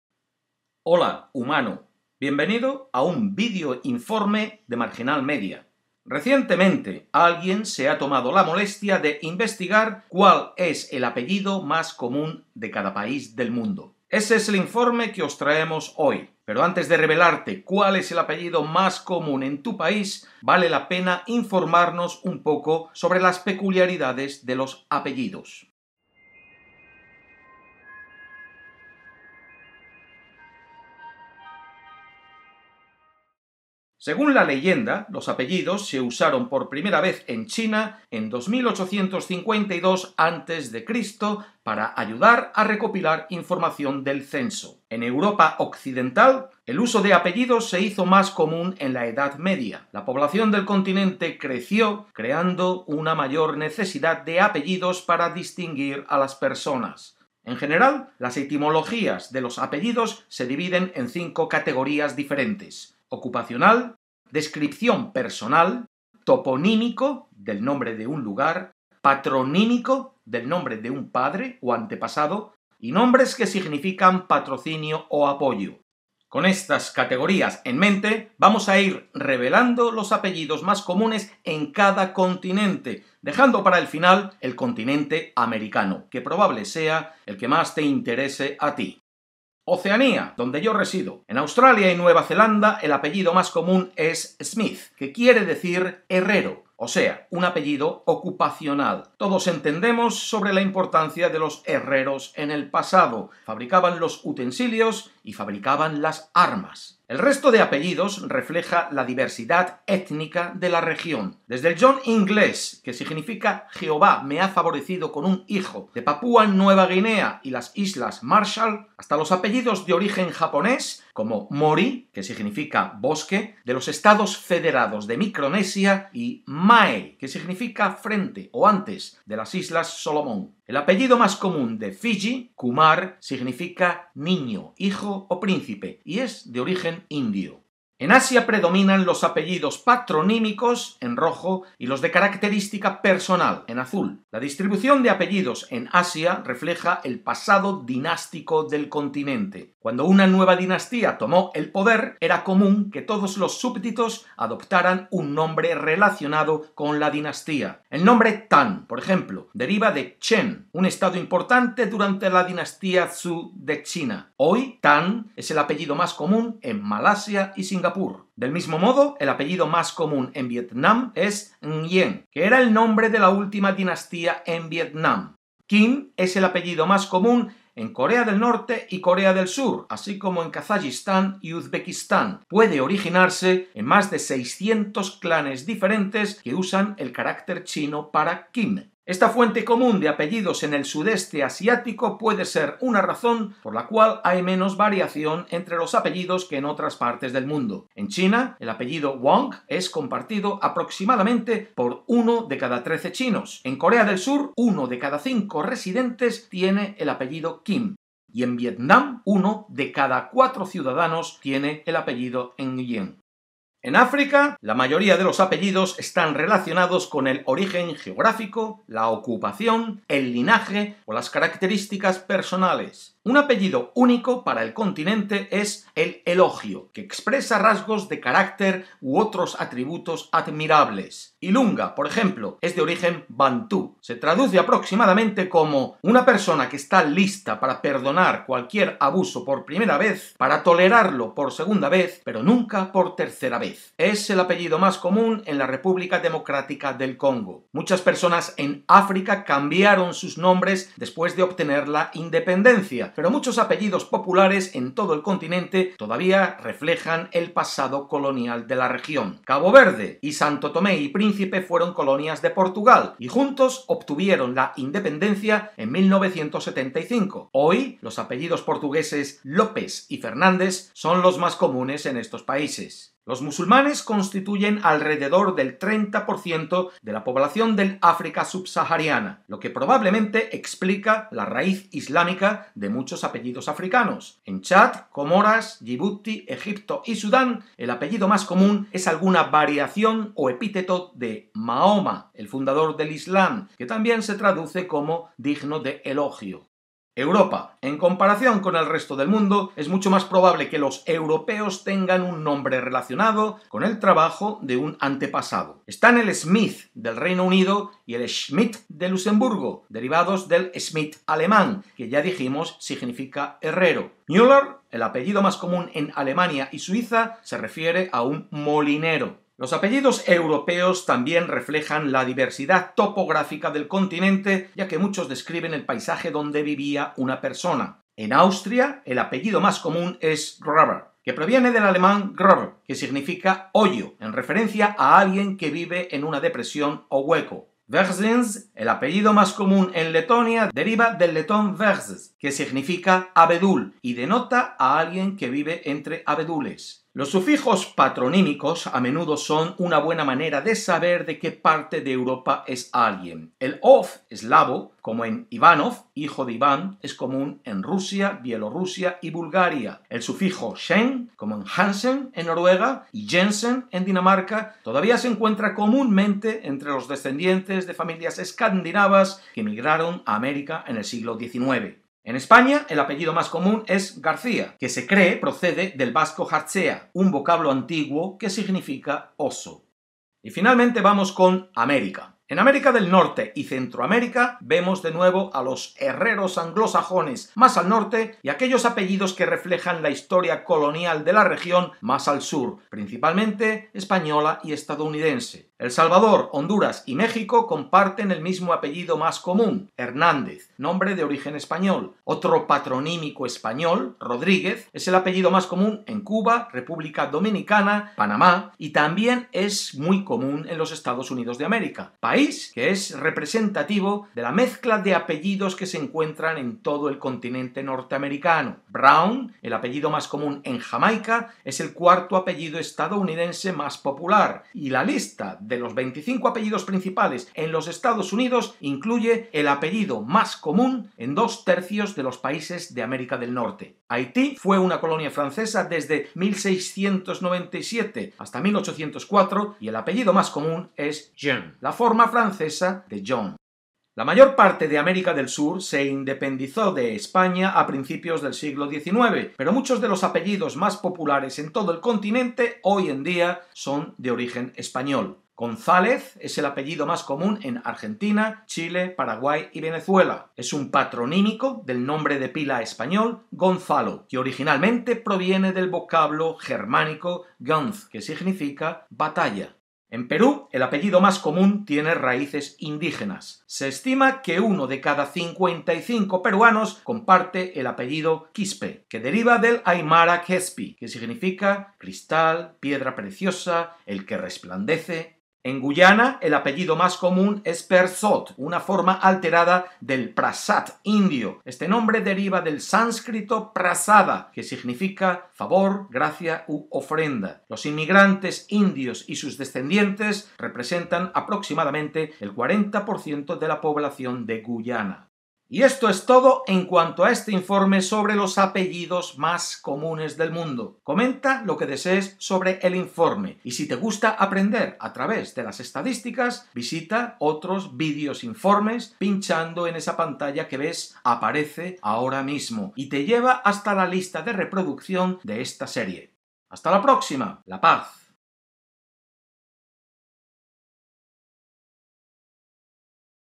Los Apellidos Más Comunes de Cada País
¿González? ¿Smith? ¿Hernández? ¿Mendes? ¿Quispe? ¿Rodríguez? ¿García? ¿Muller? ¿John? ¿Ivanov?... Un nuevo informe nos cuenta cuál es el apellido más comun en cada país.